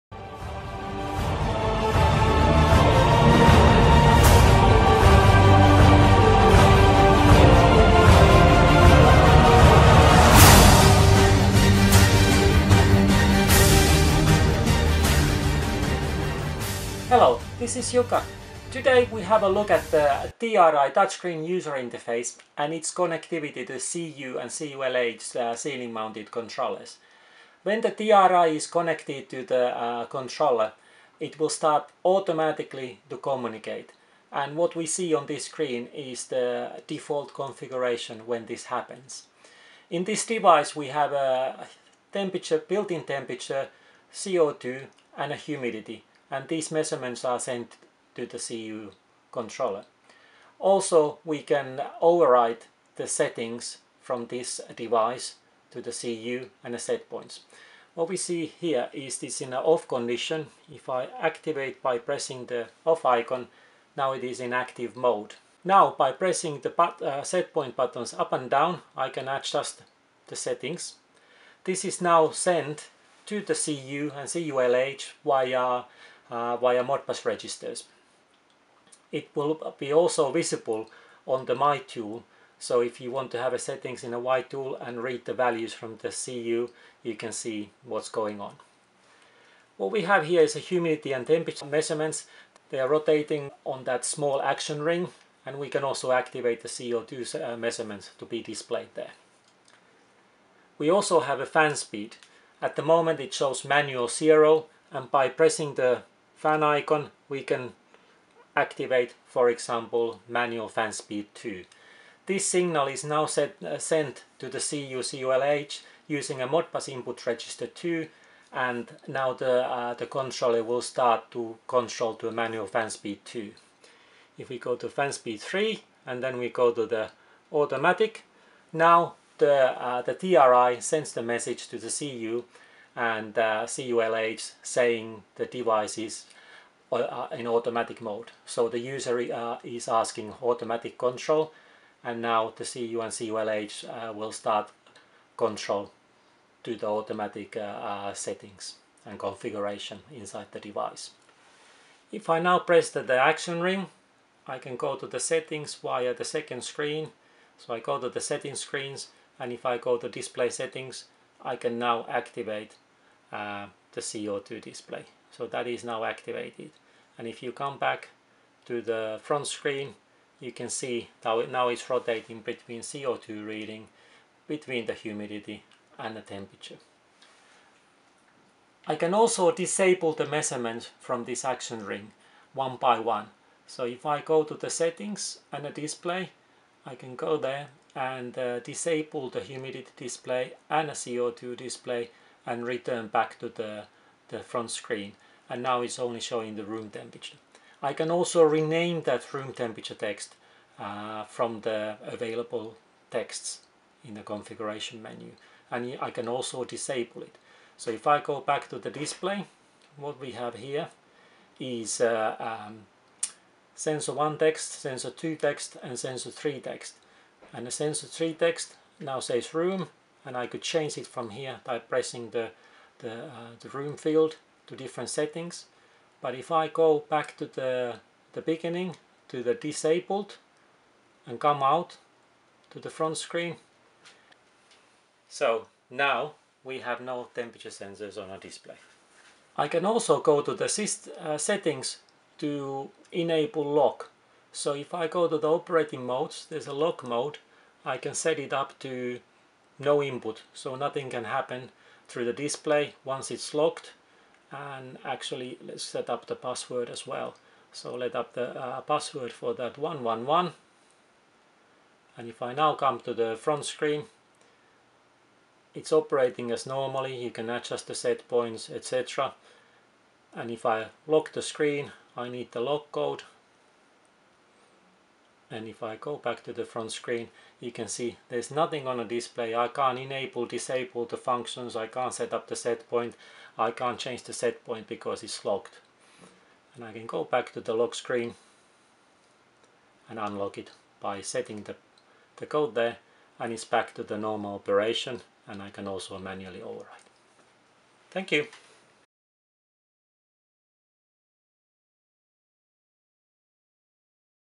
Produal TRI touchscreen and Proxima CU controller connectivity
This video demonstrates how TRI touchscreen room unit is connected to the Proxima CU and Proxima CU-LH controllers. TRI touchscreens provide attractive room interface with functionality such as:
- Temperature, Humidity and CO2 measurements
- Temperature Adjustment
- Operating Mode Adjustment
- Fan speed display and adjustment
The TRI room unit uses Modbus communication to the CU and CU-LH controllers.